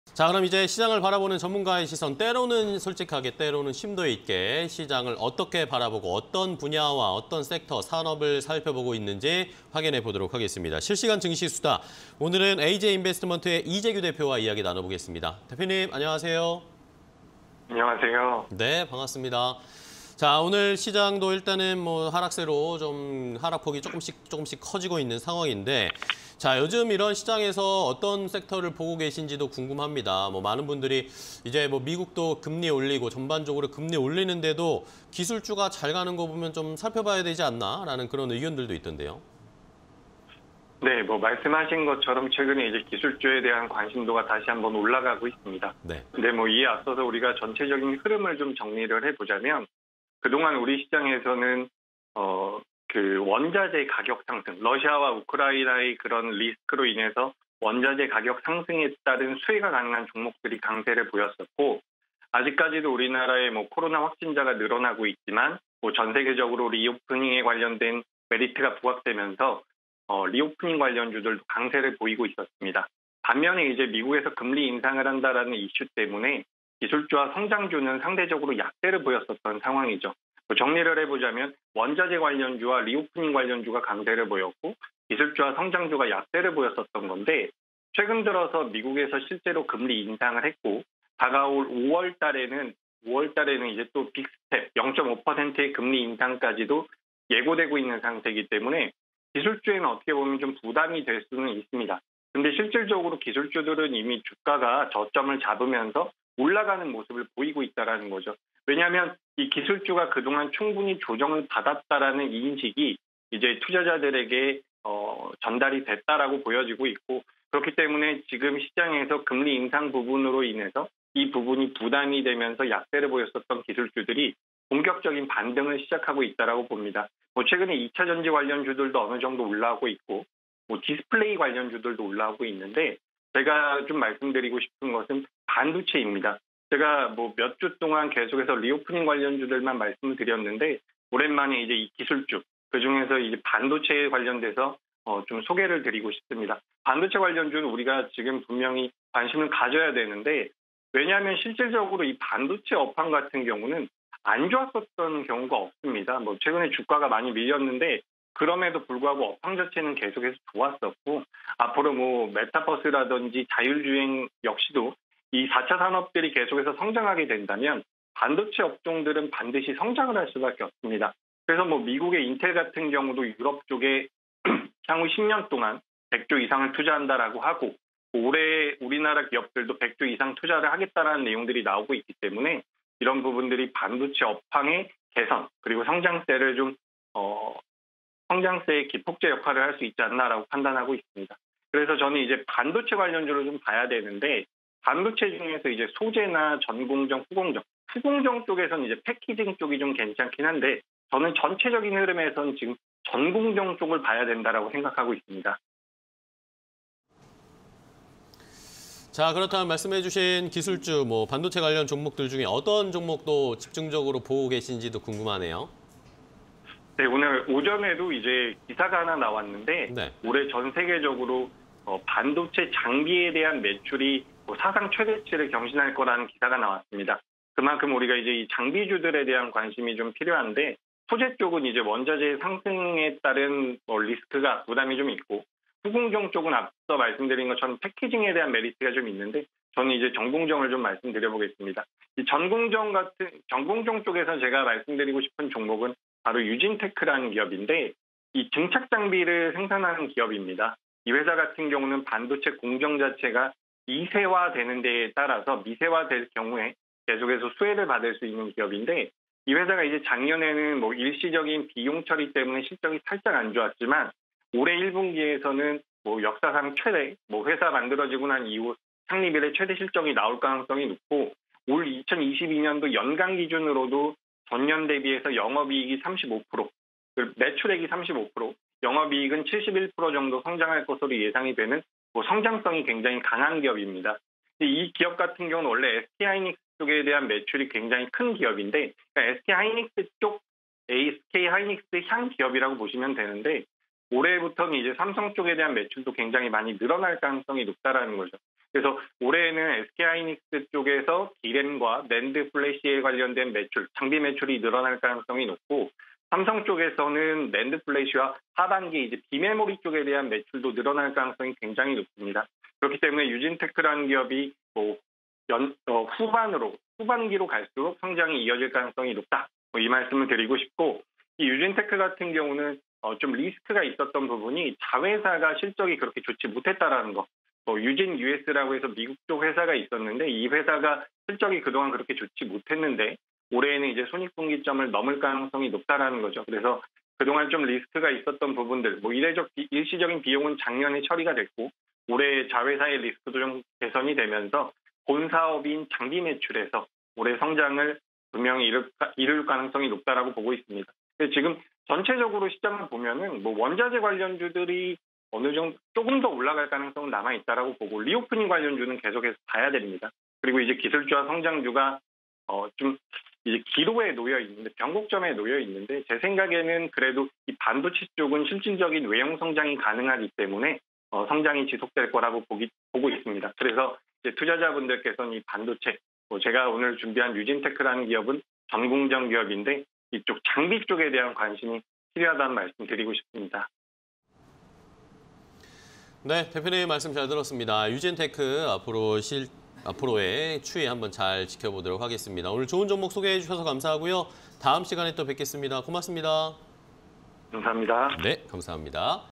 유진테크(084370)... 기술주 강세 속 반도체 투자 확대_실시간 증시 수다 (20220324)
Q. 기술주, 상승 랠리 이어가나?
- 美 금리 인상 및 러-우크라 전쟁 악재 지속
- 글로벌 증시에서 '기술주-성장주' 반등 시작
- 주가 하락에 악재 선반영 분석
- 반도체 관련주 비롯 성장주 관심 필요
- 메타버스·자동차 등 시장서 반도체 수요 증가

유진테크(084370)
- 전공정의 증착장비 생산 업체
- 반도체 공정 미세화시 수혜 가능
- DDR4-DDR5 전환 등 실적 기대감↑
- 자회사 유진US, 적자폭 축소 가능성↑
- D램·낸드·비메모리까지 장비 매출 다각화